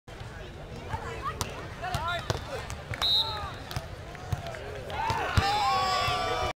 Beach Volleyball Manhattan Beach AVP 2009
AVP professional beach volleyball clips. Techniques and strategies for how to play beach volleyball. Learn by watching the pros in action!